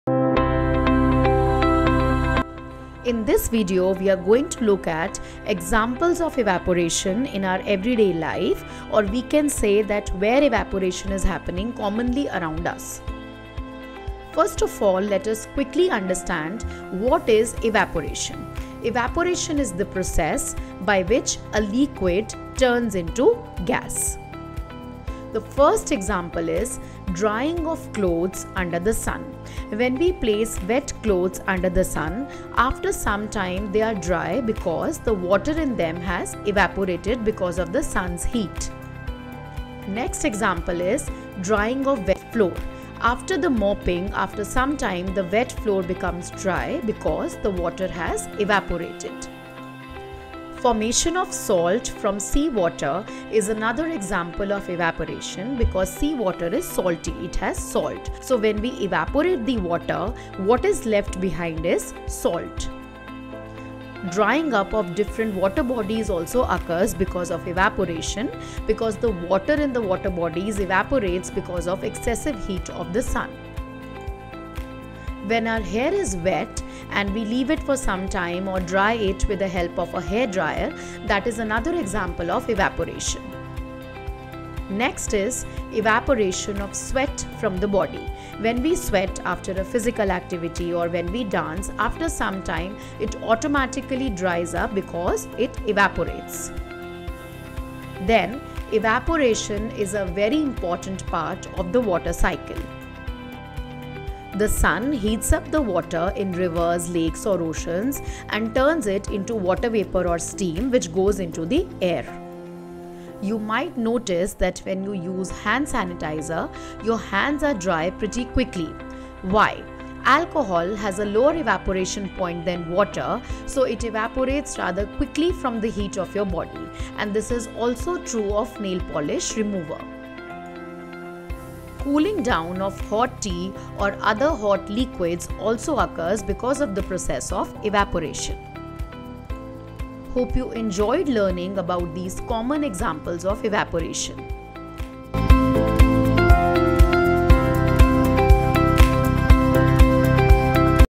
Common Examples Of Evaporation | Evaporation Around Us | Simple Examples Explained
Do you know evaporation is all around us . Curious about how water mysteriously disappears without boiling? 🌊 Join us as we uncover the secrets of evaporation in our everyday lives!

From drying clothes to vanishing sanitizer, we'll explore common examples of this natural process. 🌞 Discover the science behind why your wet laundry dries on the line or why a sweat disappears after some time. It's all about evaporation, and we'll break it down for you in this simple, easy-to-understand video. 💧